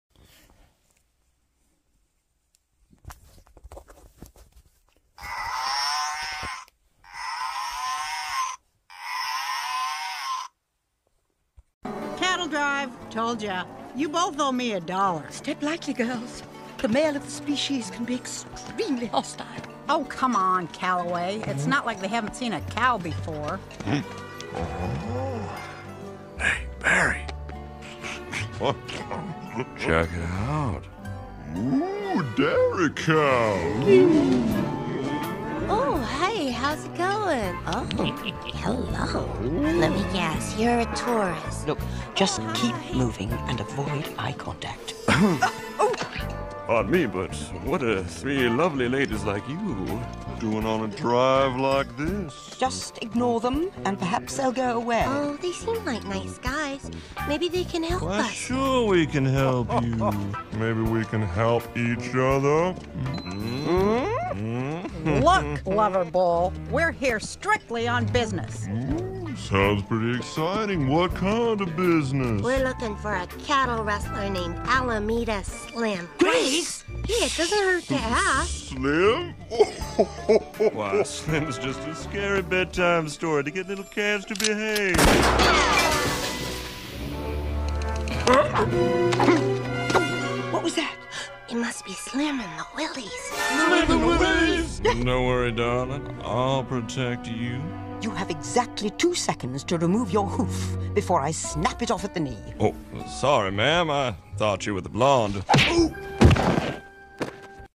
Maggie' Mrs.Calloway And Grace (Plush Toy Version) From Disney's Home On The Range.
It's The (Plush Toy Version) Of Maggie' Mrs.Calloway And Grace.
This Is Maggie' Mrs.Calloway And Grace He Says Moo 🐮.
In This The Dairy Cows 🐄 In Got Milk 🥛.
From Disney's Home On The Range.
For The Walt Disney Company.